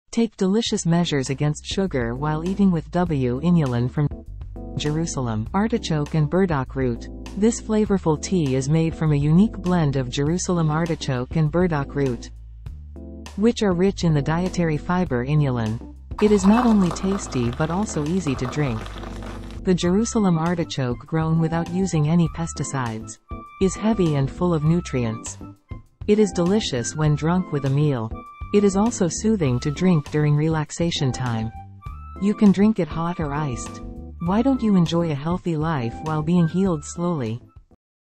Jerusalem Artichoke Burdock Tea PV
For those of you who tend to lack fiber in your diet.
This nutrient-rich tea contains inulin, a type of soluble dietary fiber, and minerals.

Deliciously made using only Japanese Jerusalem artichoke and burdock root.
Since it is caffeine-free, you can drink it anytime without worrying about the time.
※15 tea bags per package